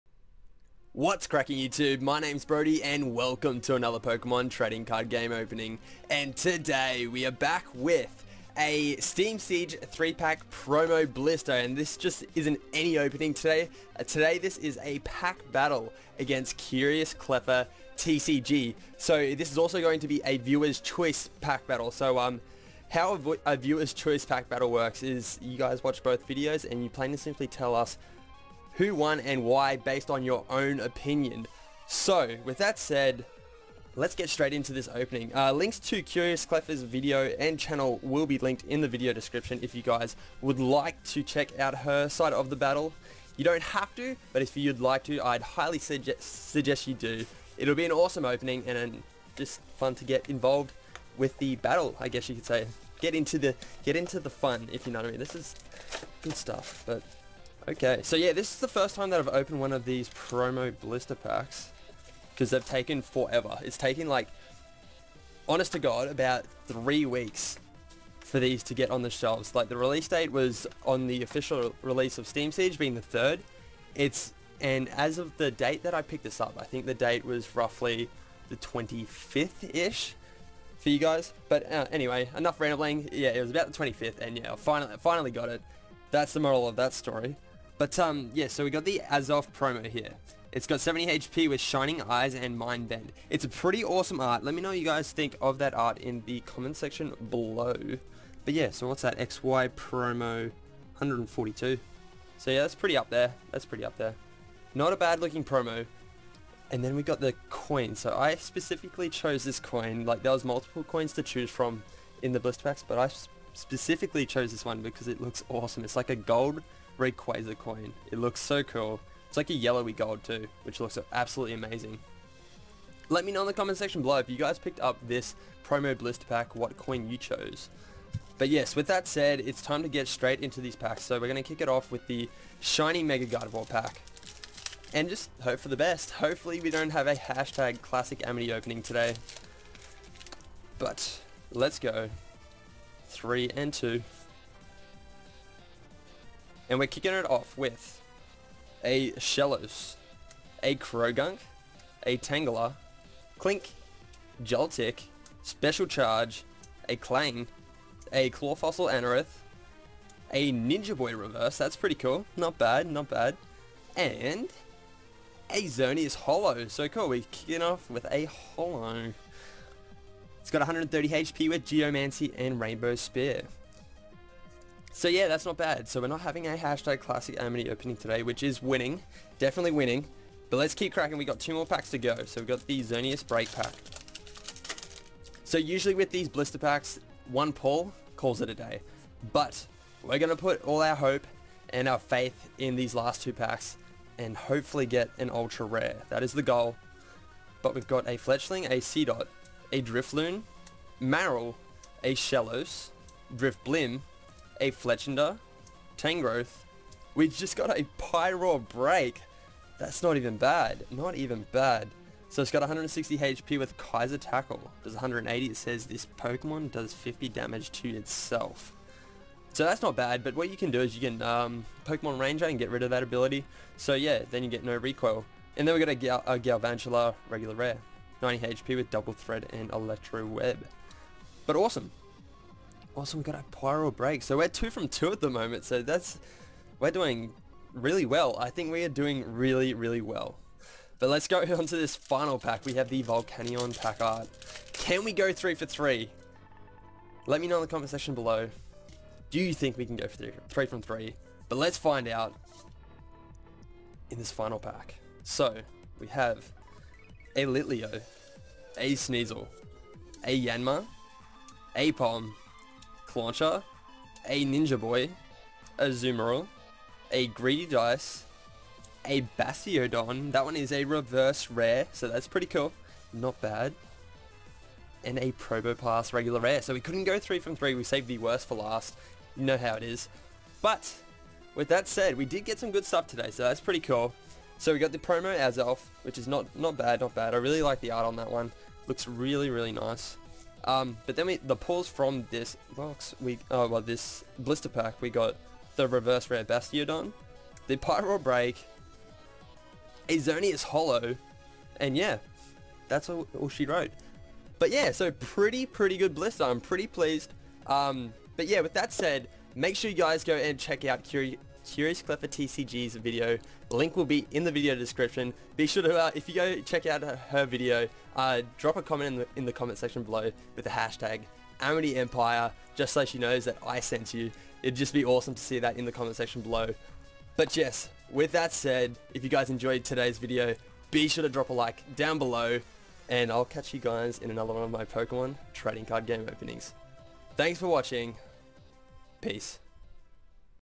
Steam Siege Azelf 3 Pack Promo Blister Opening! Vs. CuriousCleffaTCG
What's Crackin' YouTube! Today we open up one of the new promo blister packs released with the Steam Siege Expansion! This time around being the Azelf promo, this opening is also a pack battle against CuriousCleffaTCG so be sure to check out her channel and side of the battle!

You read this far into the description...?!
You are a true member of the AmityEmpire